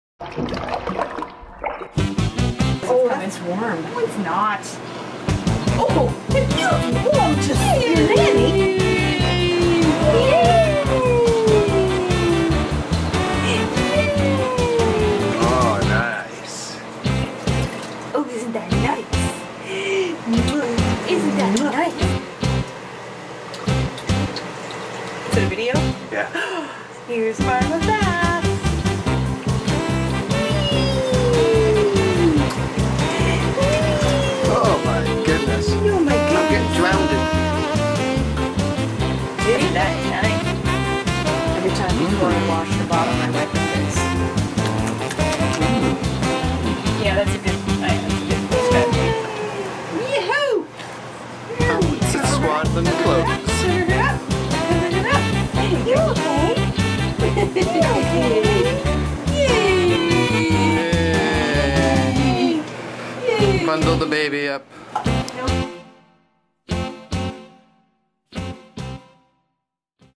Margot Apr 20
Margot narrowly escapes falling into the washing machine to have a bath with mom and nana.